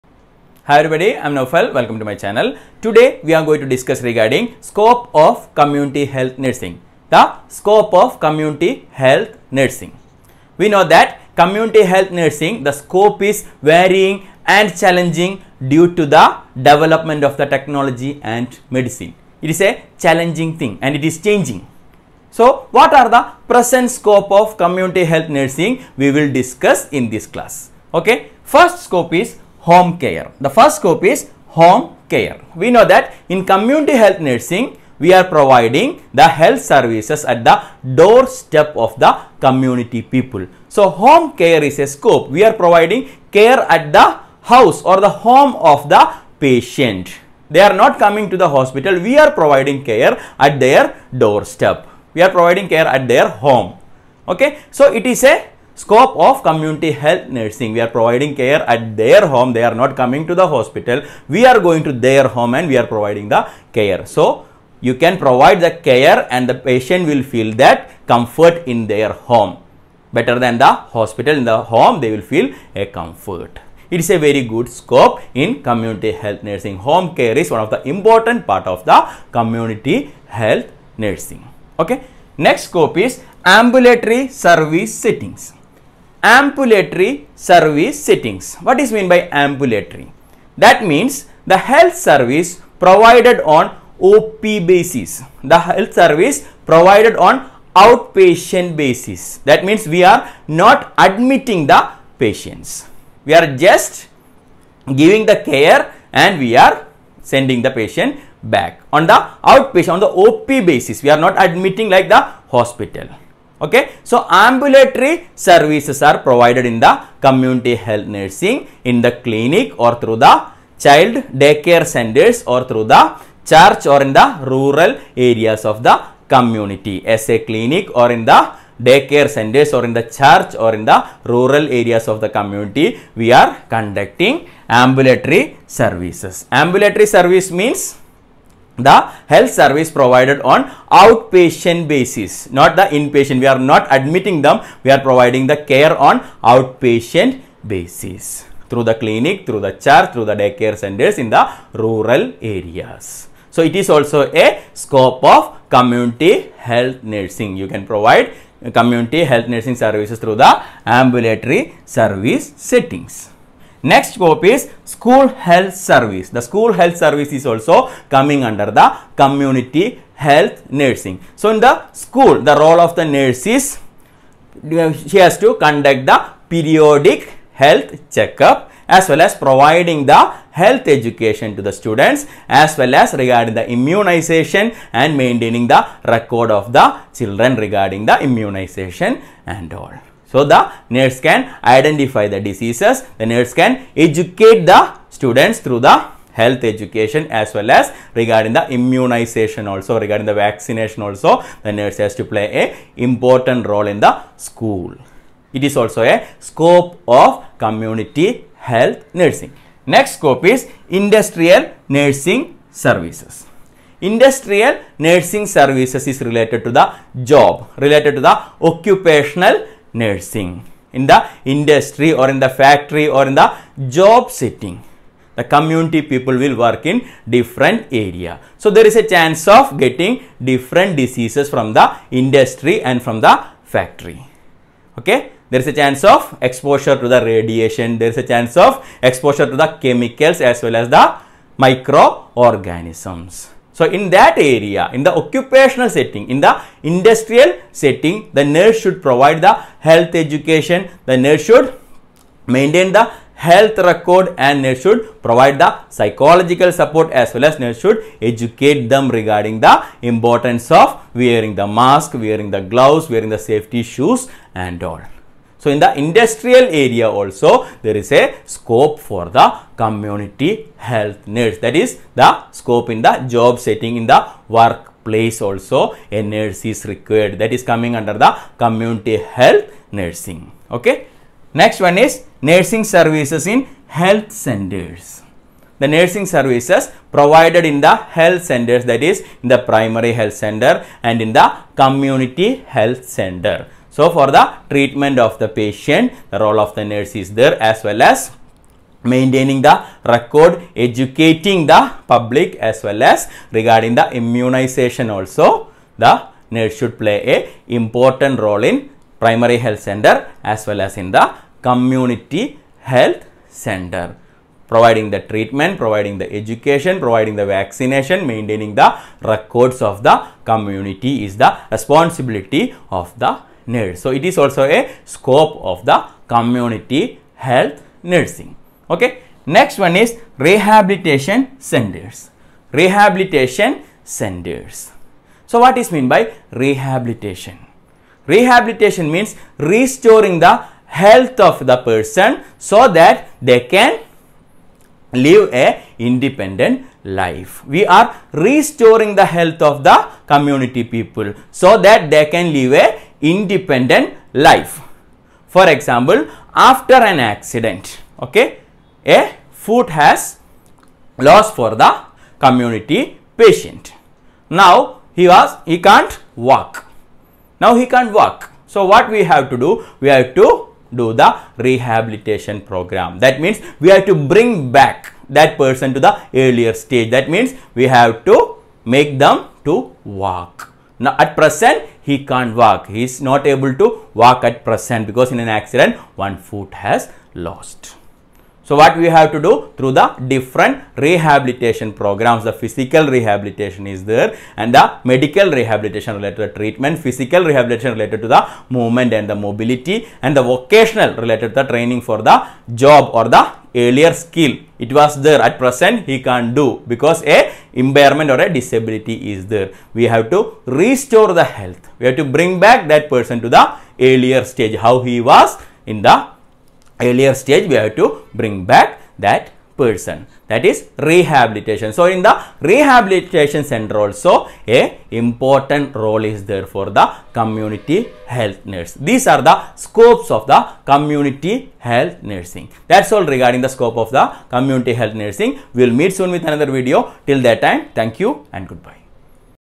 Scope of community health nursing/ English
Easy and simple explanation of scope of community health nursing with examples in English